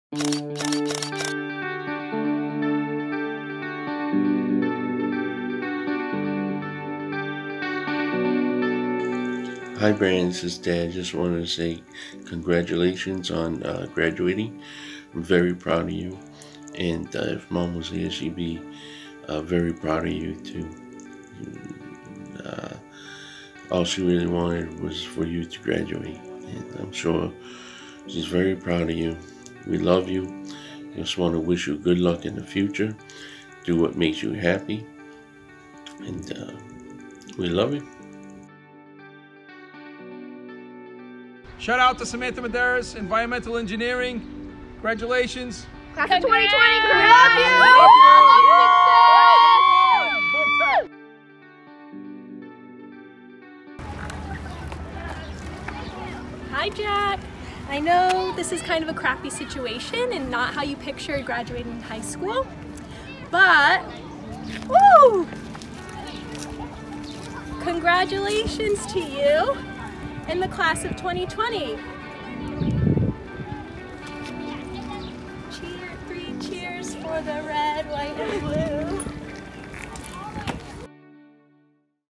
Senior Shoutouts: Episode 2
Tuesday, May 19th - Today for senior shoutouts, Brandin Jefferson, Samantha Medeiros and Jack Gifford.